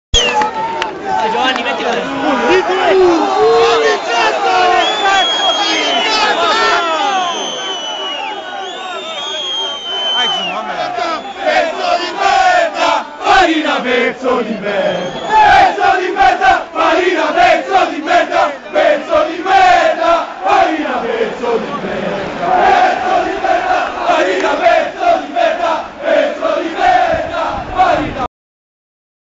reggina-livorno 31/10/07 farina pezzo di merda
la curva sud di reggio intona un coro contro farina, l'arbitro del match